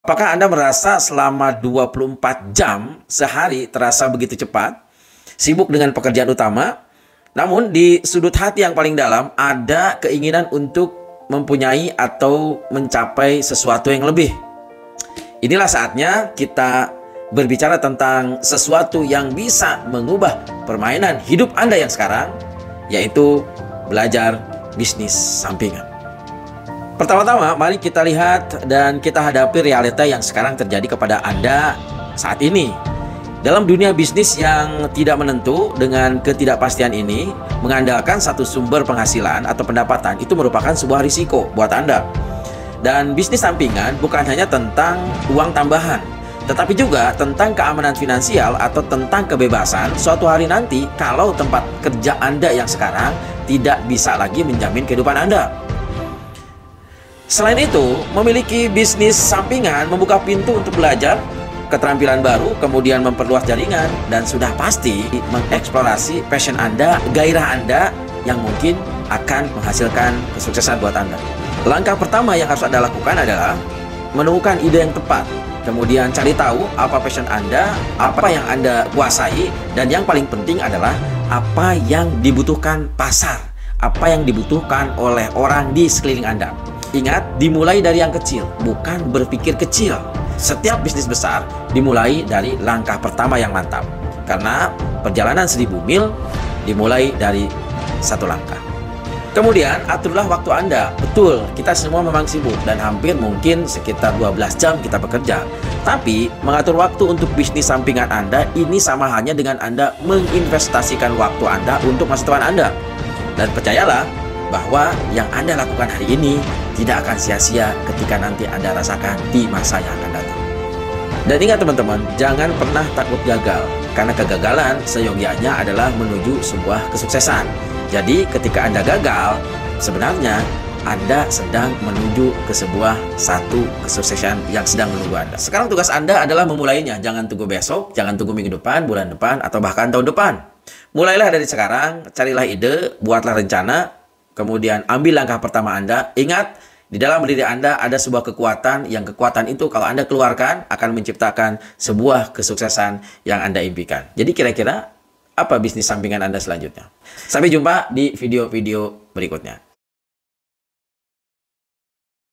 Bagaimana Memulai Bisnis Sampingan
Kenapa Punya Bisnis selaiin pekerjaan utama itu penting ? Saksikan video singkatnya disini.